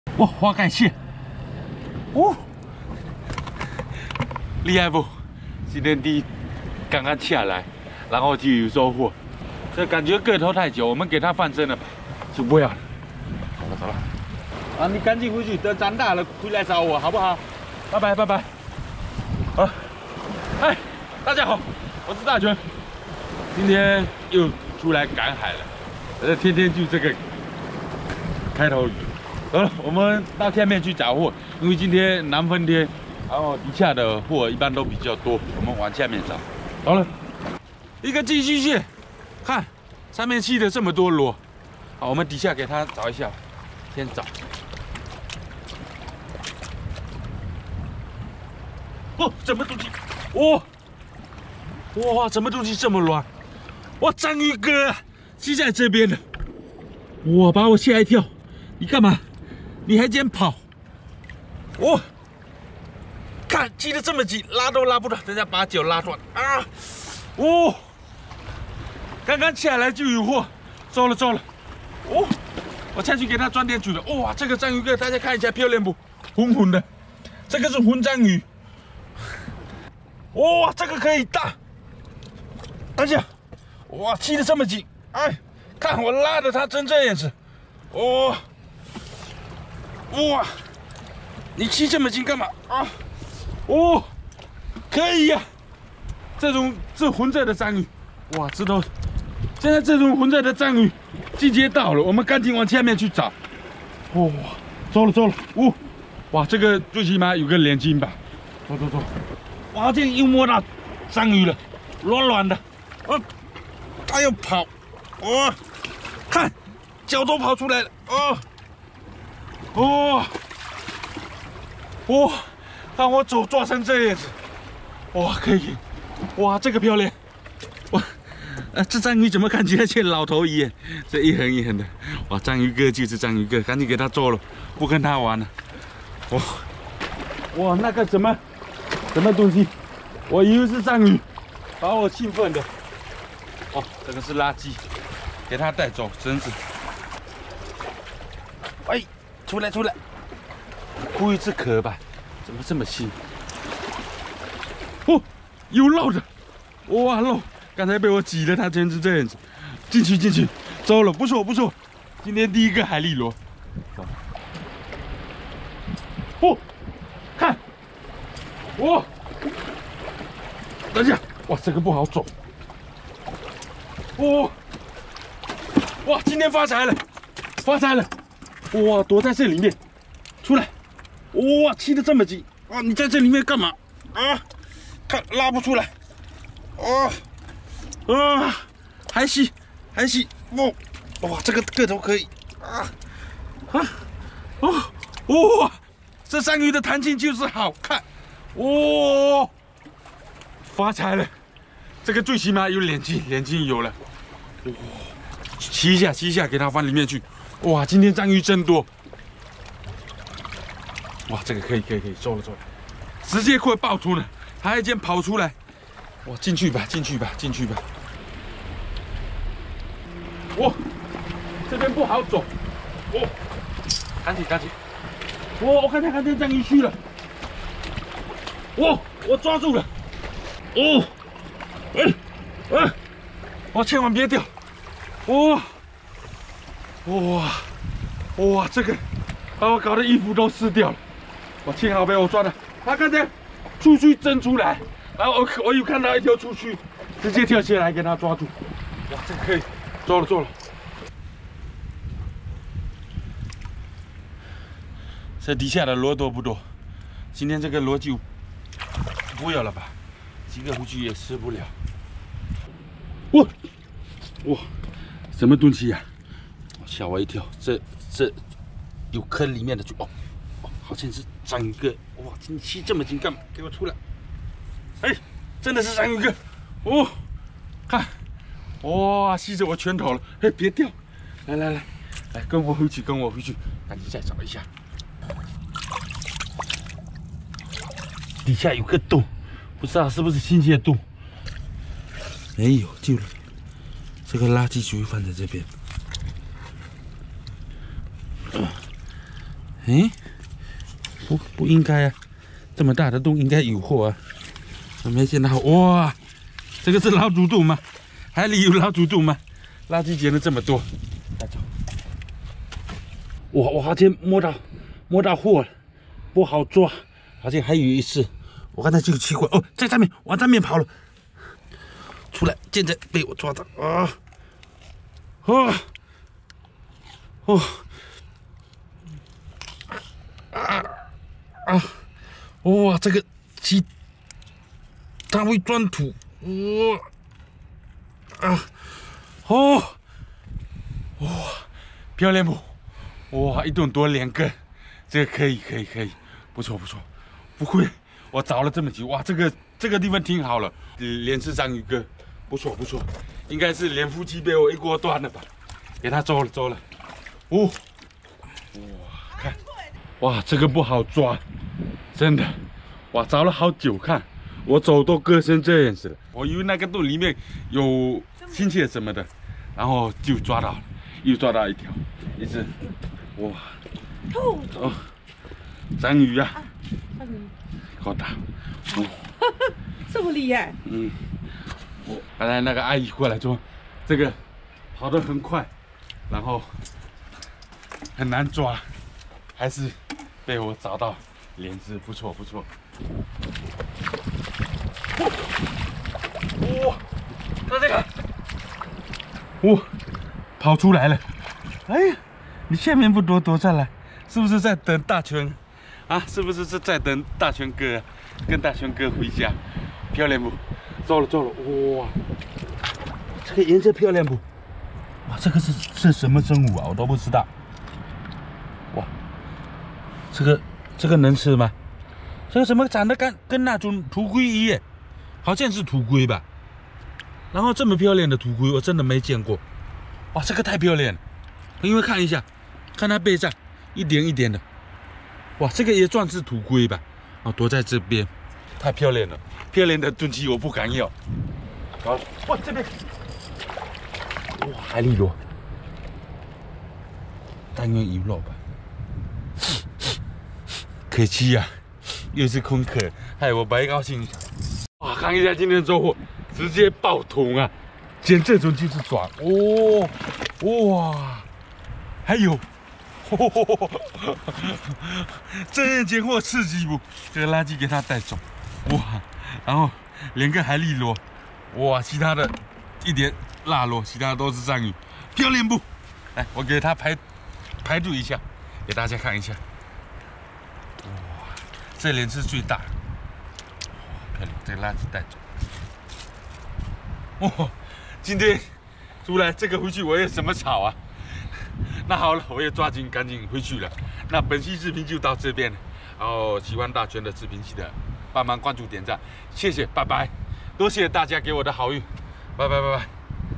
南風天章魚家族集體上岸，兩夫妻被大熊一鍋端，一隻比一隻大
【兄弟趕海】南風天章魚家族集體上岸，兩夫妻被大熊一鍋端，一隻比一隻大